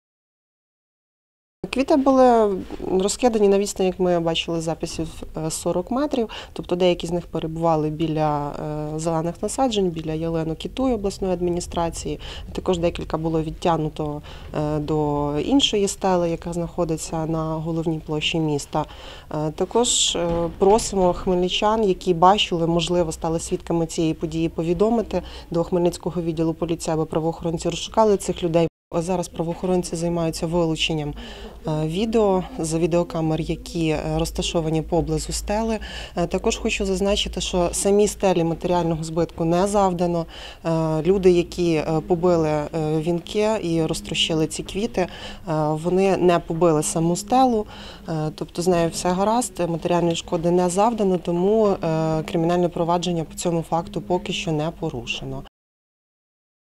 Вандали у Хмельницькому коментар
Вночі вандали поглумилися над «Стелою пам`яті загиблим героям Небесної сотні та воїнам АТО», що у Хмельницькому на Майдані Незалежності. Невідомі розтрощили вінки і лампадки, котрі напередодні принесли небайдужі городяни, вшановуючи загиблих під час Революції Гідності.